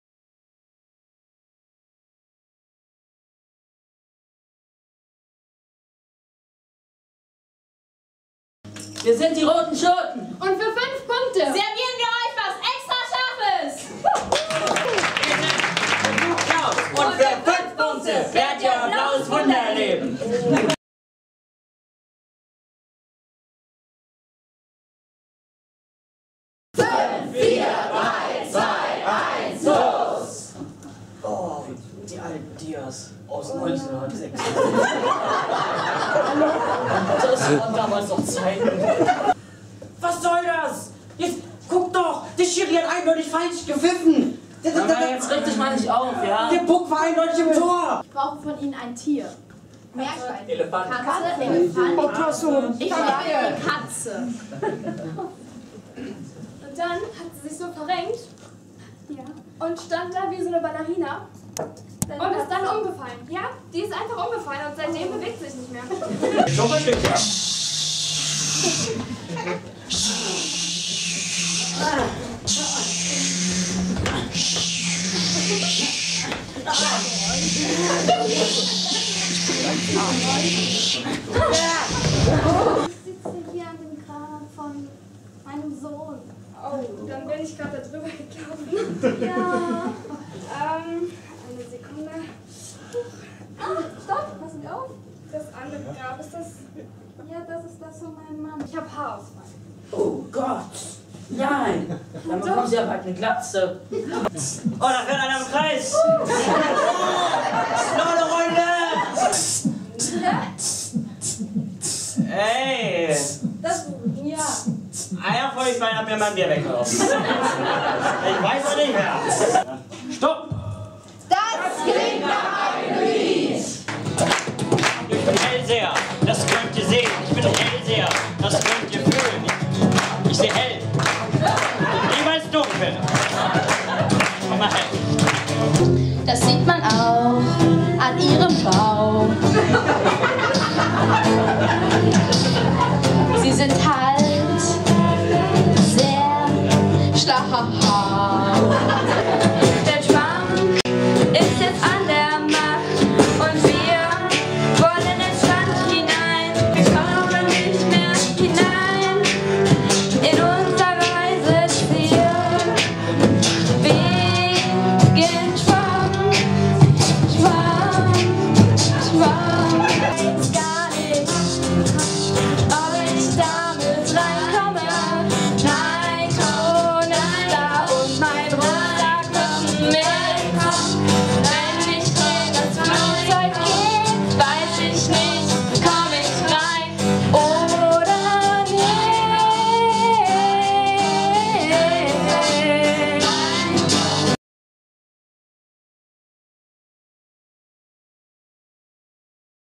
theater MINESTONE Theatersport
Ein kleiner Ausschnitt aus dem ersten
TheaterSportAbend in Wülfrath am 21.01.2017
mit dem jungen Ensemble "Chilis" des theater MINESTRONE